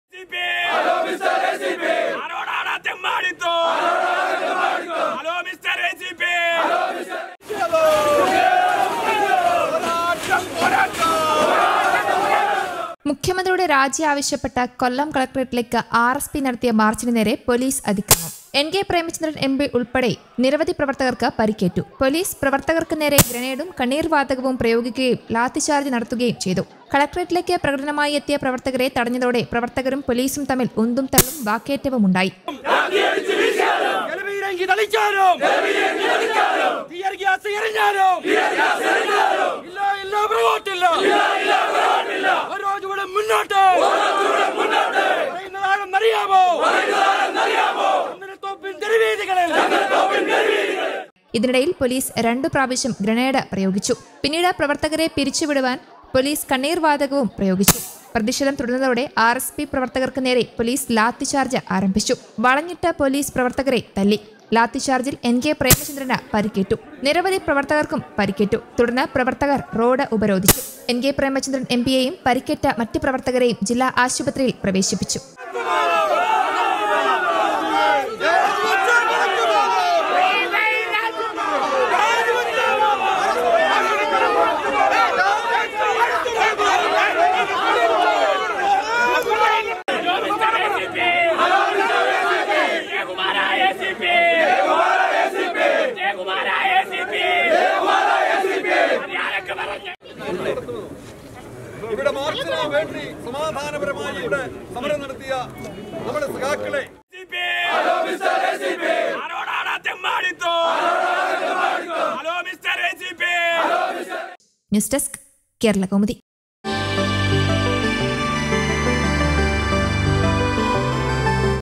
കൊല്ലത്ത് R.S.P പ്രതിഷേധത്തില്‍ സംഹാര താണ്ഡവമാടി പൊലീസ് | RSP March | Kollam | Kerala Police
മ്യഖ്യമന്ത്രിയുടെ രാജി ആവശ്യപ്പെട്ട് കൊല്ലം കളക്ടറേറ്റിലേക്ക് ആര്‍.എസ്.പി നടത്തിയ മാര്‍ച്ചിന് നേരെ പോലിസതിക്രമം . എന്‍. കെ.പ്രേമചന്ദ്രന്‍ എം.പി ഉള്‍പ്പെടെ നിരവധി പ്രവര്‍ത്തകര്‍ക്ക് പരിക്കേറ്റു. പോലീസ് പ്രവര്‍ത്തകര്‍ക്ക് നേരേ ഗ്രനേഡും കണ്ണീര്‍ വാതകവും പ്രയോഗിക്കുകയും ലാത്തി ചാര്‍ജ് നടത്തുകയും ചെയ്തു.കളക്ടറേറ്റിലേക്ക് പ്രകടനമായി എത്തിയ പ്രവര്‍ത്തകരെ തടഞ്ഞതോടെ പ്രവര്‍ത്തകരും പോലിസും തമ്മില്‍ ഉന്തുംതള്ളും വാക്കേറ്റവുമുണ്ടായി. ഇതിനിടയില്‍ പോലിസ് രണ്ട് പ്രാവശ്യം ഗ്രനേഡ് പ്രയോഗിച്ചു. പിന്നീട് പ്രവര്‍ത്തകരെ പിരിച്ച് വിടുവാന്‍ പോലിസ് കണ്ണീര്‍ വാതകവും പ്രയോഗിച്ചു.